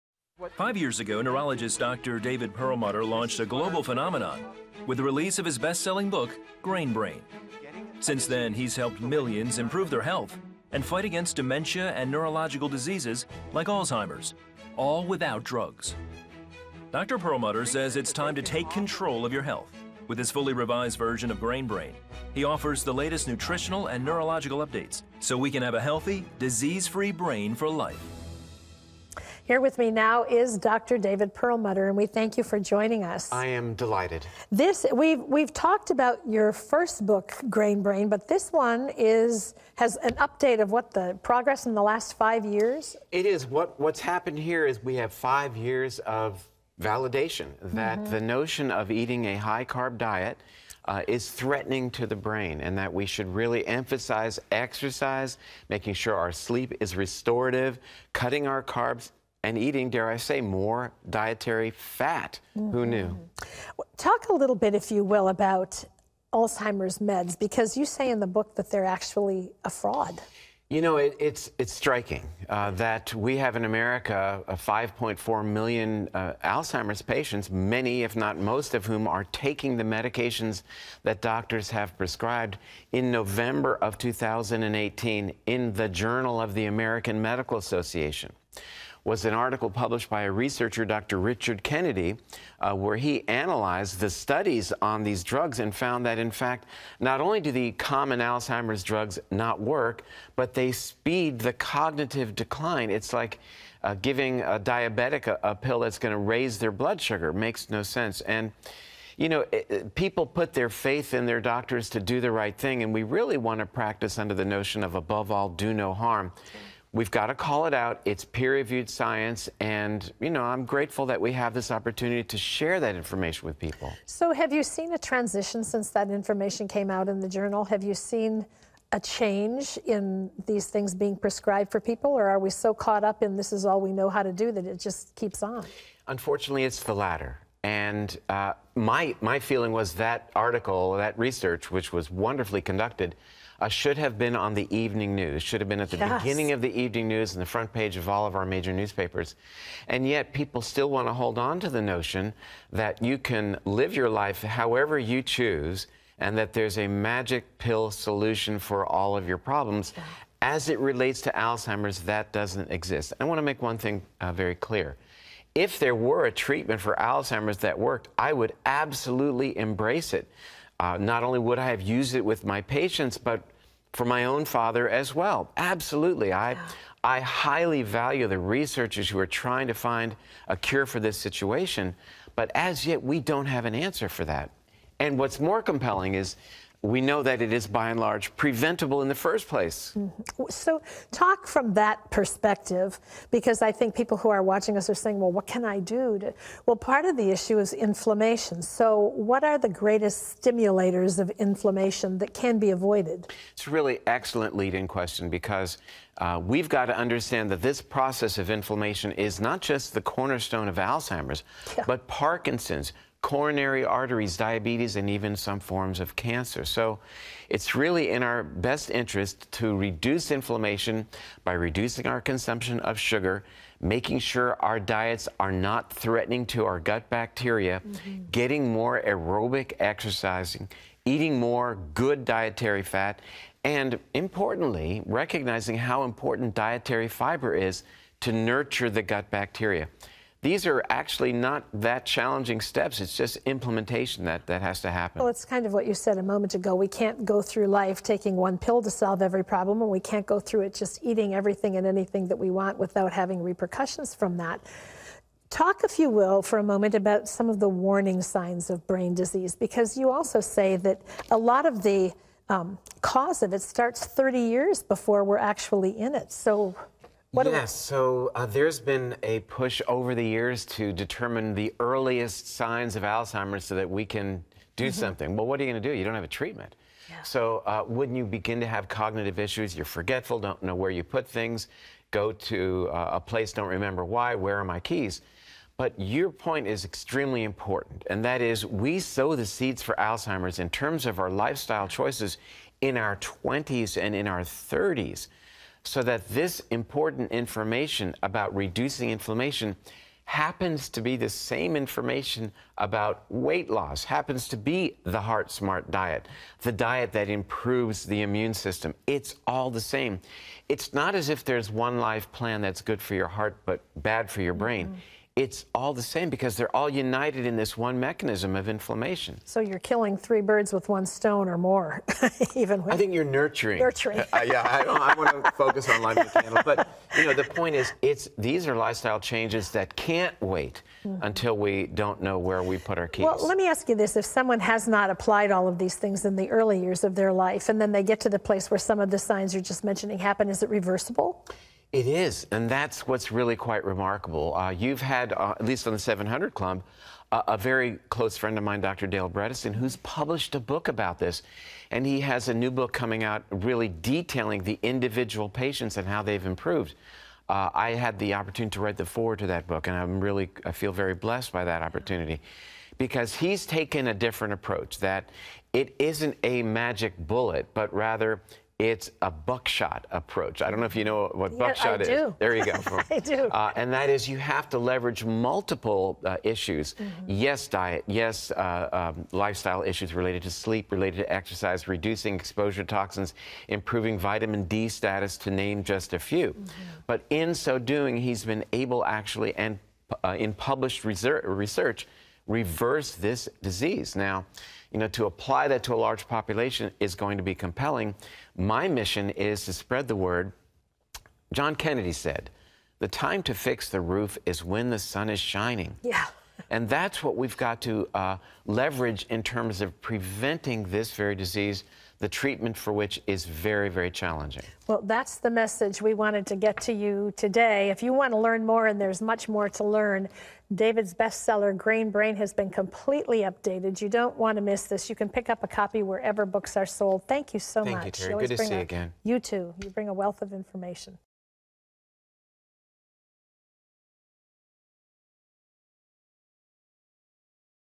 Dr. David Perlmutter | The Grain Brain | Identifying Your Brain’s Silent Killers
Dr. David Perlmutter will discuss the surprising truth about wheat, carbs and sugar- your brain’s silent killers.